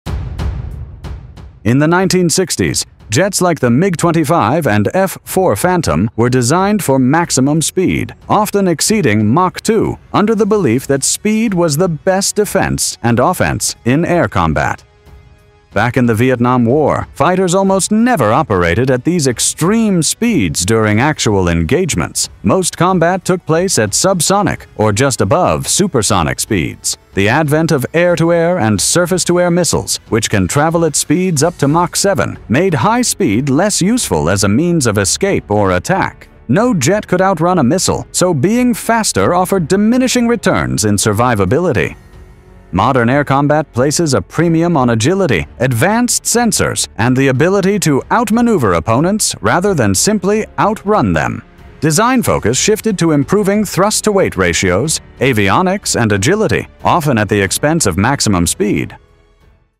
Why Modern Fighter Jets Slower Than '60s?
In the 1950s and 1960s, speed was seen as a critical advantage, especially for intercepting enemy bombers or escaping threats. Aircraft like the MiG-25 and SR-71 were designed for extreme speed, with the MiG-25 reaching over Mach 2.8 and the SR-71 over Mach 3.3